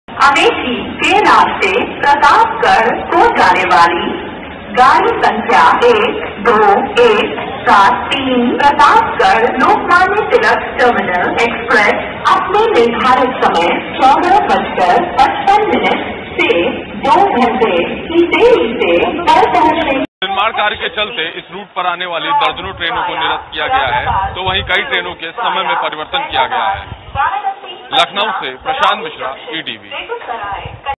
लखनऊ : यात्री गढ़ कृपया ध्यान दें, पटरी की मरम्मत की वजह से ट्रेनें प्रभावित
लखनऊ : मानक नगर रेलवे स्टेशन पर पटरी के मरम्मत का कार्य चल रहा है. इसके चलते इस स्टेशन से होकर लखनऊ रेलवे स्टेशन पहुंचने वाली लगभग दर्जनभर ट्रेनों को निरस्त कर दिया गया है तो वहीं कई ट्रेनों के रूट में परिवर्तन किया गया है.